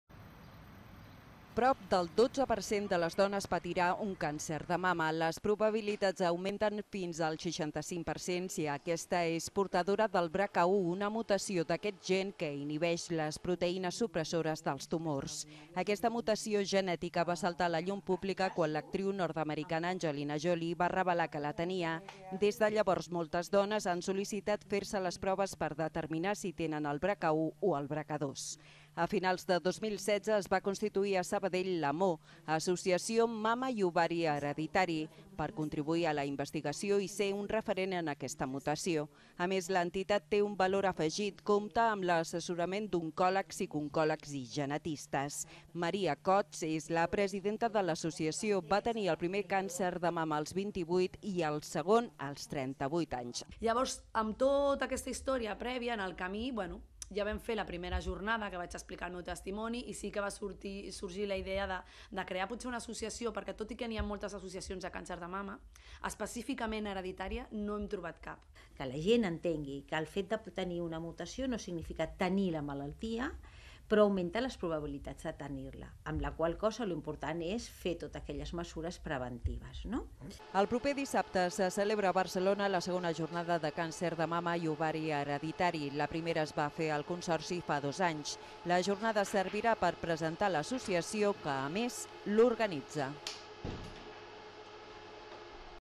Neix AMOH, una associació que vol ser referent en el càncer de mama i ovari hereditari
Prop del 12% de les dones patiran un càncer de mama. Les probabilitats augmenten fins al 65% si és portadora del BRCA1, una mutació d’aquest gen que inhibeix les proteïnes supressores dels tumors.
Aquesta mutació genètica va saltar a la llum pública quan l’actriu nord-americana Angelina Jolie va revelar que la tenia.
El juny de 2016 es va constituir a Sabadell l’AMOH (Associació Mama&Ovari Hereditari) per contribuir a la investigació i ser un referent en aquesta mutació.